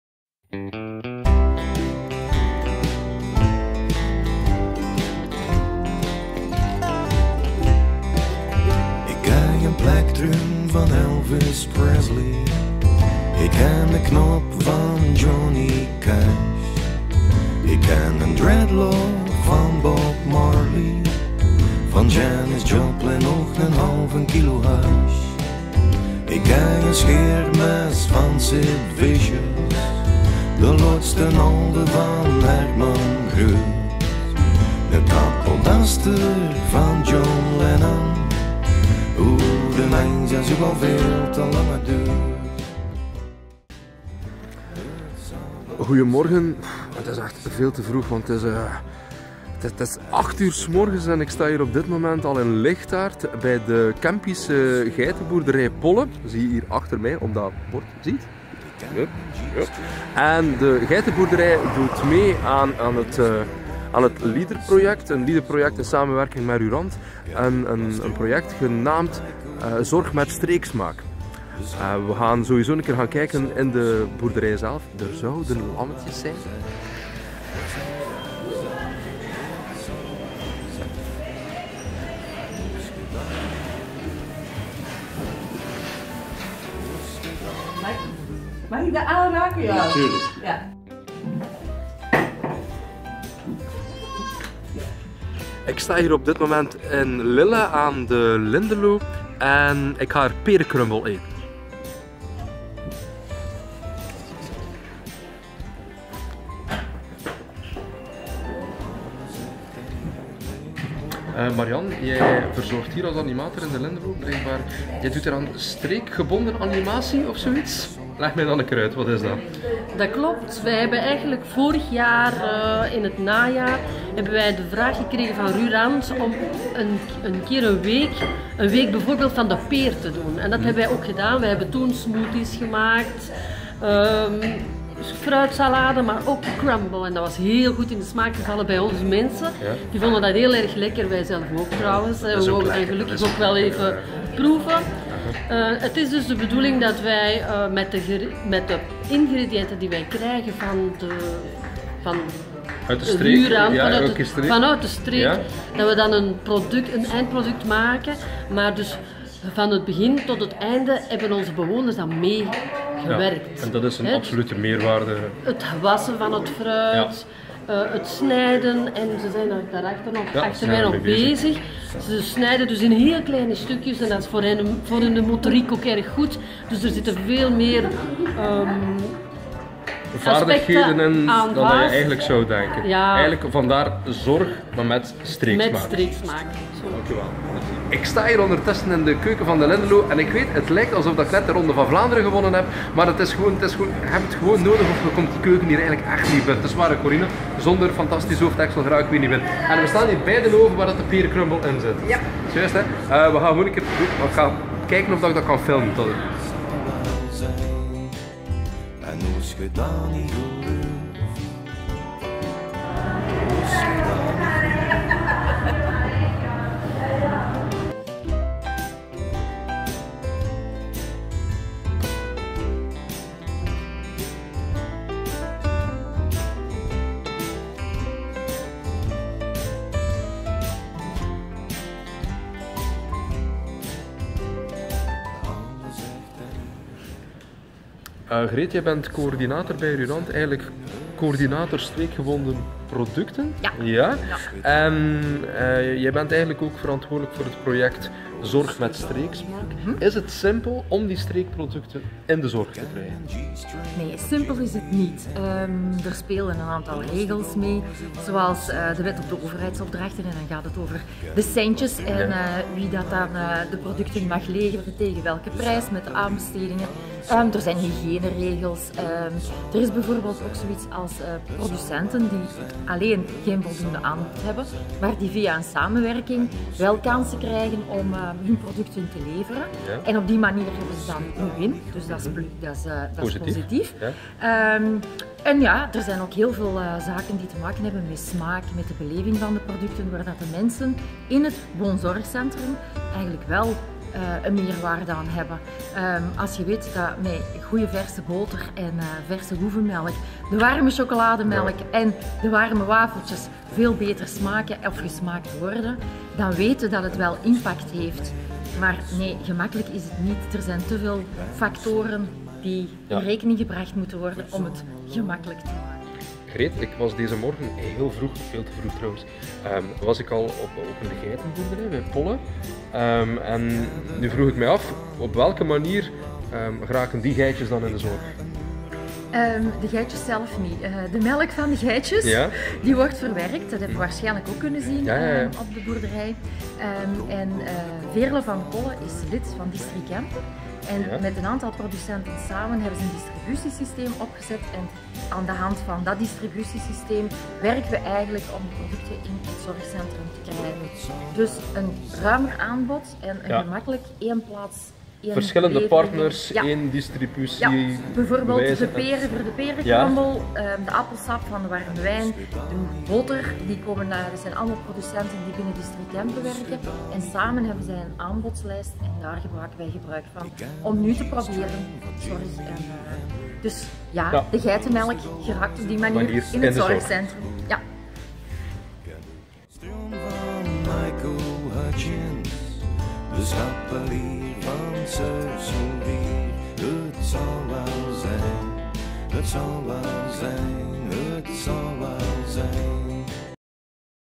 Zorg met streeksmaak
LEADER - project waarin met streekproducten in de Zorg probeert te brengen.